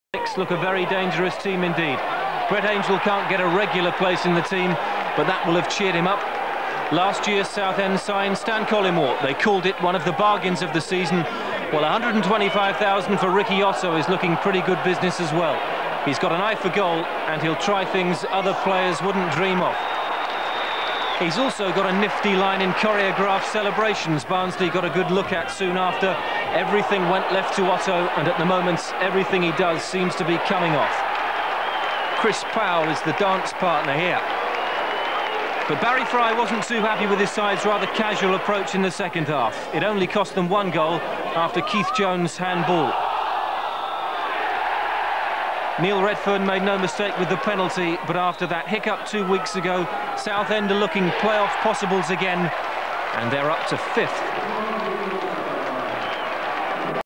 Barnsley v Southend United 23rd October 1993 - Division 1 - Otto's brilliant lob - highlights
23rd October 1993
Endsleigh League Division 1

Barnsley 1-3 Southend United
Attendance: 5,240

Southend United team
1. Simon ROYCE
2. Gary POOLE
3. Chris POWELL
4. Andy EDWARDS
5. Pat SCULLY
6. Jonathan HUNT
7. Andy ANSAH
8. Phil GRIDELET
9. Tommy MOONEY
10. Ricky OTTO
11. Brett ANGELL
12. Keith JONES
13. Ron FEARON
14. Jason LEE

Goalscorers: ANGELL, OTTO (2)